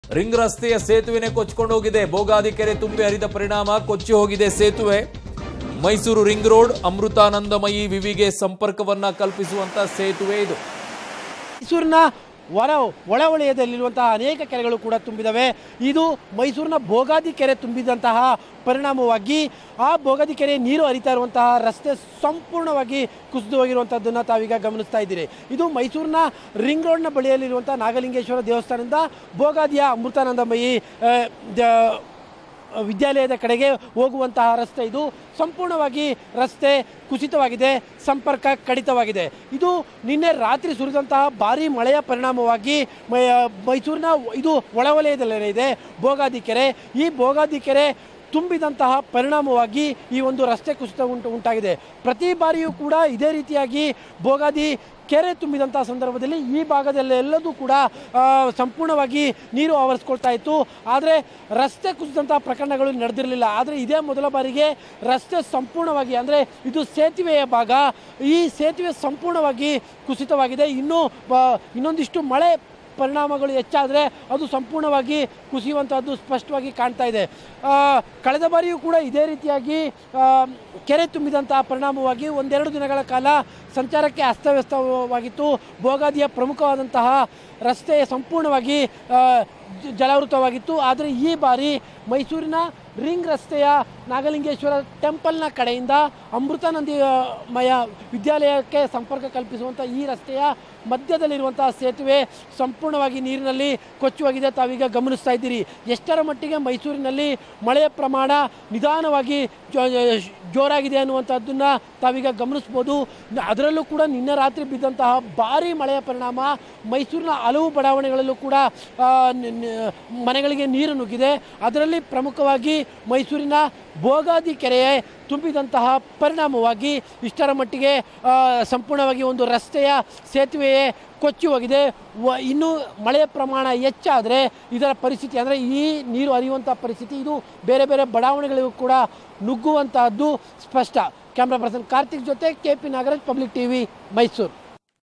Bridge Washed Away Due To Heavy Rain In Mysuru | Bogadi Lake
ಮೈಸೂರಿನಲ್ಲಿ ನಿನ್ನೆ ರಾತ್ರಿ ಭಾರೀ ಮಳೆ
ಕೊಚ್ಚಿ ಹೋಯ್ತು ರಿಂಗ್ ರಸ್ತೆಯ ಅರ್ಧ ಸೇತುವೆ
ಬೋಗಾದಿ ಕೆರೆ ತುಂಬಿ ಕೊಚ್ಚಿ ಹೋದ ಸೇತುವೆ
ಬೋಗಾದಿ ನಾಗಲಿಂಗೇಶ್ವರ ದೇವಸ್ಥಾನದ ಬಳಿ ಘಟನೆ.
ಮೈಸೂರು ರಿಂಗ್‍ರೋಡ್- ಅಮೃತಾನಂದಮಯಿ ವಿವಿಗೆ ಸಂಪರ್ಕ ಸೇತುವೆ